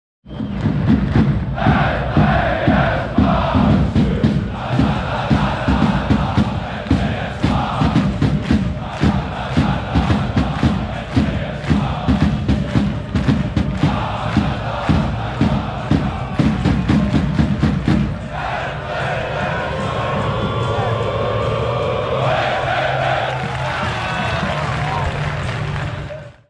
AGF - Brøndby: Andreas Maxsø fenerbahce (24.11.2019)
Fenerbahce inden Brøndby skulle møde AGF den 24. november 2019

Video: Teis Markfoged